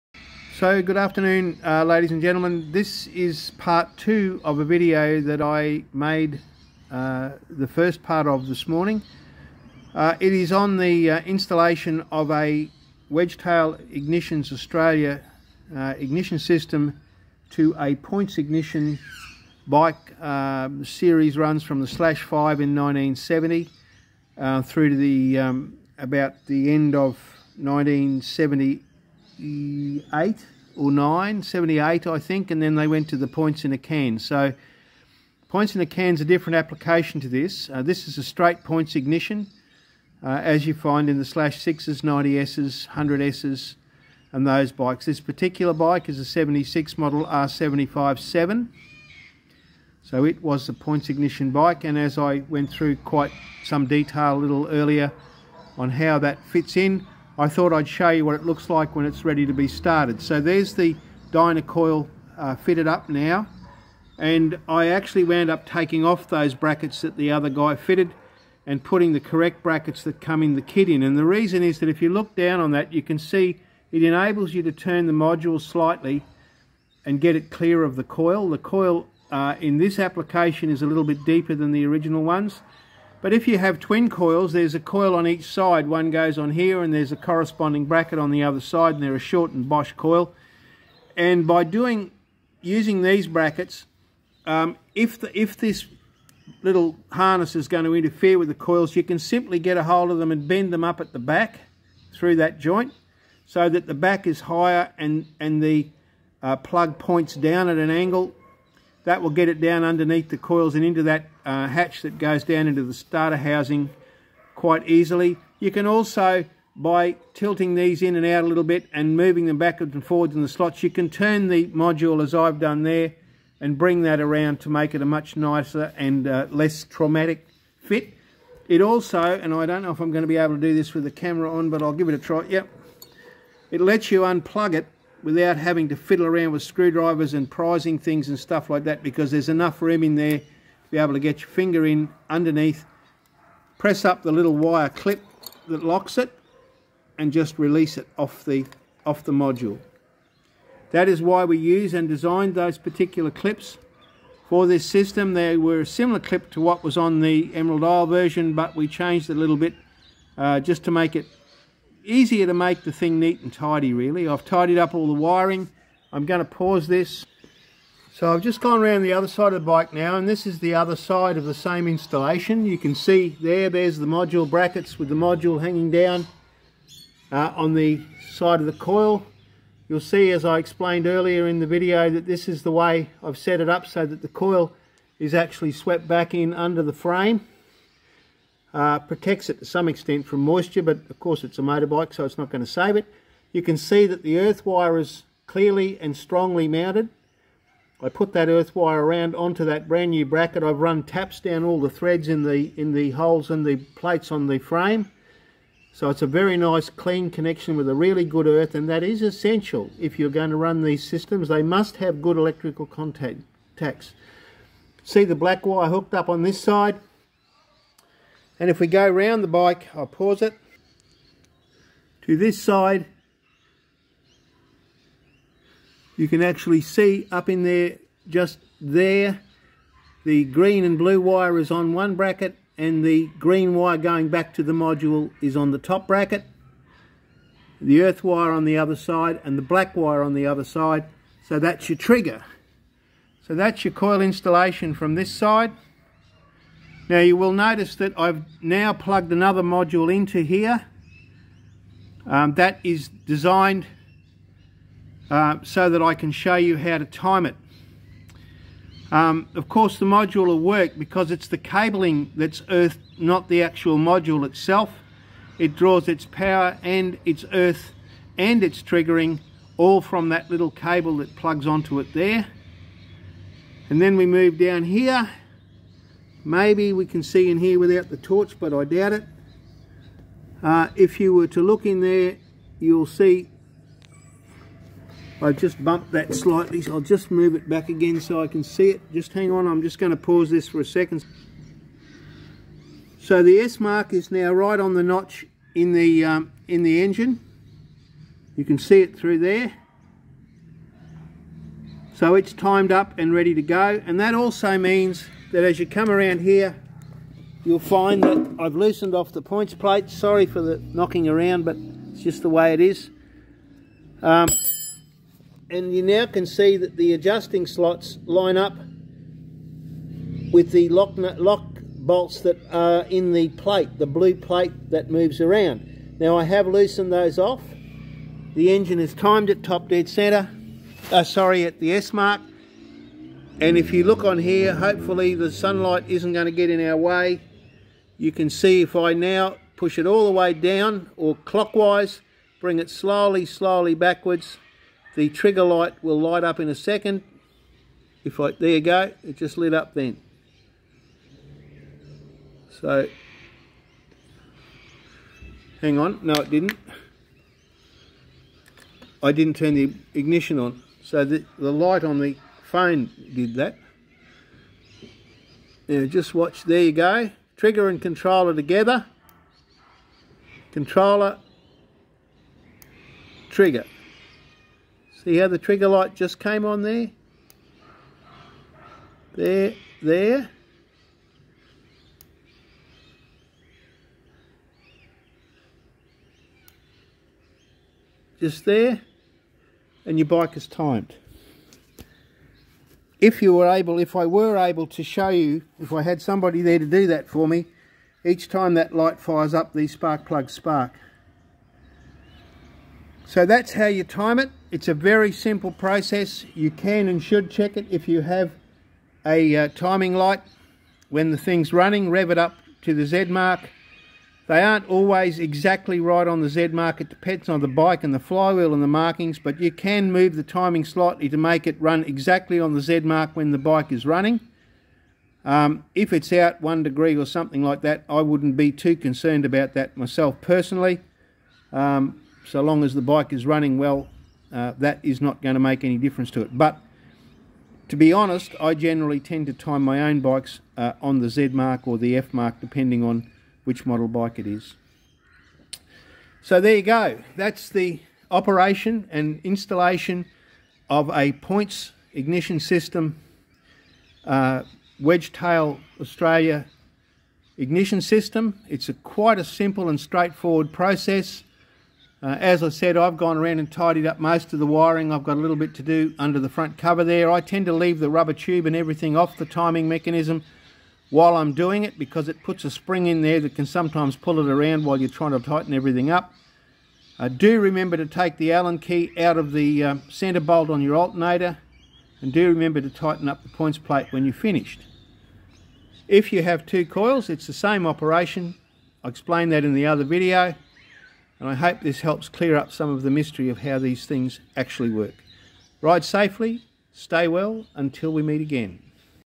WEDGETAIL IGNITIONS AUSTRALIA Installation Video fo BMW Airheads POINTS IIGNITION UPGRADE PART 2
In this video we complete the instructions on the complete installation of the AUSTRALIAN DESIGNED AND MADE WEDGETAIL IGNITION SYSTEM